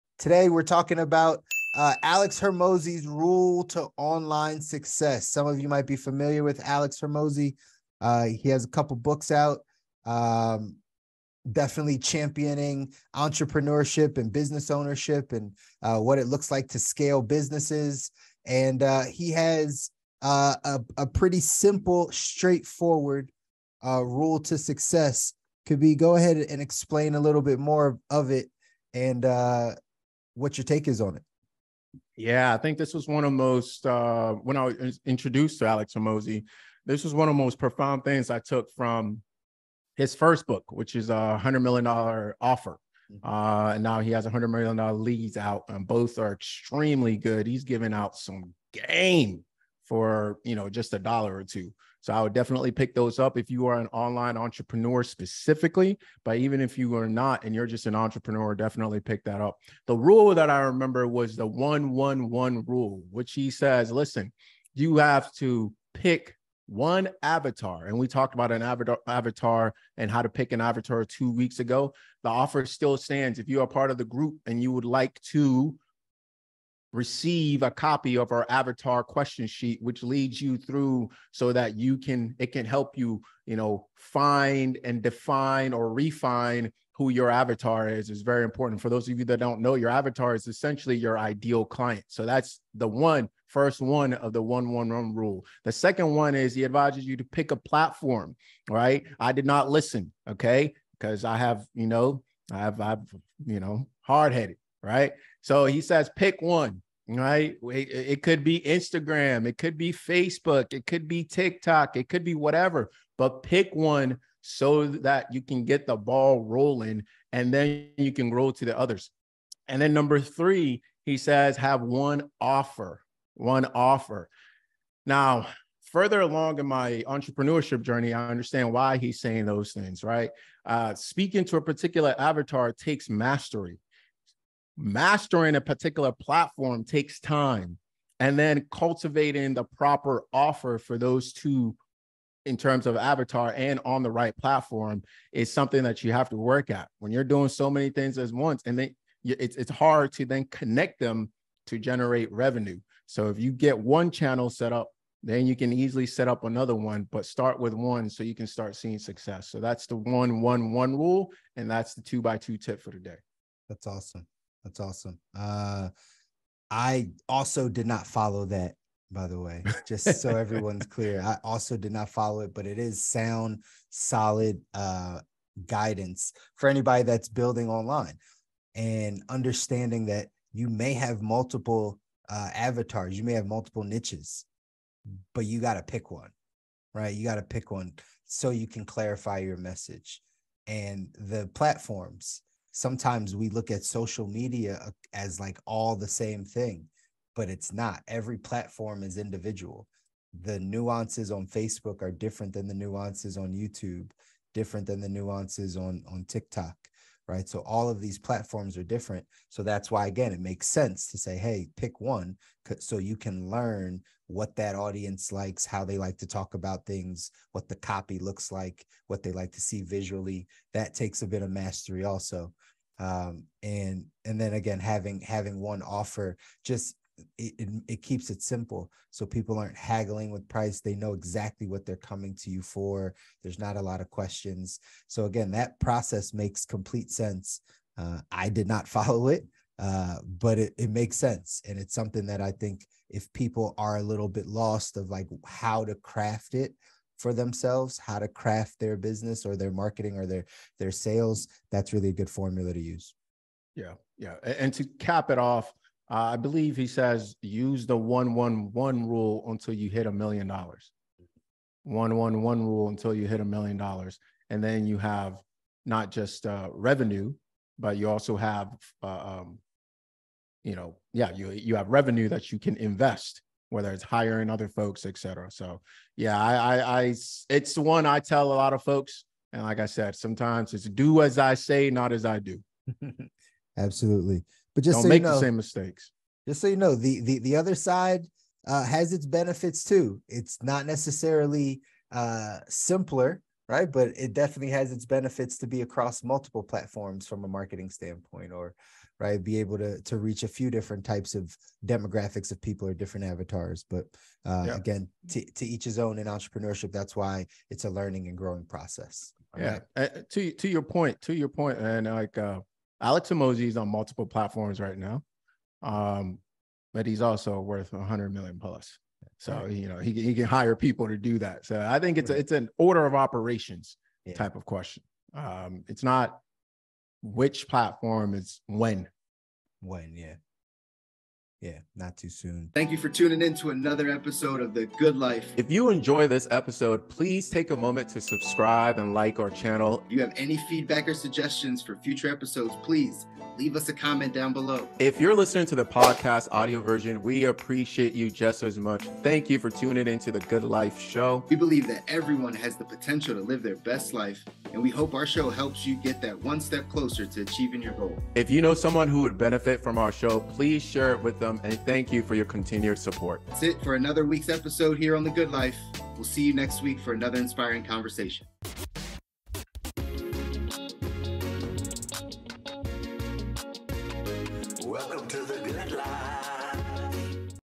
This is Alex Hormozi's Rule to Online Success
Join us as we dive deep into the world of online success with the renowned entrepreneur, Alex Hormozi. In this video, we're going to share Alex Hormozi's rule to online success and how you can use it to take your business to the next level.

Discover the secrets, strategies, and mindset that drive success in the digital age. Whether you're a seasoned entrepreneur or just starting your online journey, Alex Hormozi's insights will inspire, guide, and supercharge your path to success.

Join our private community for exclusive content on optimizing your health and wellness:

We are now available on your favorite podcast platforms!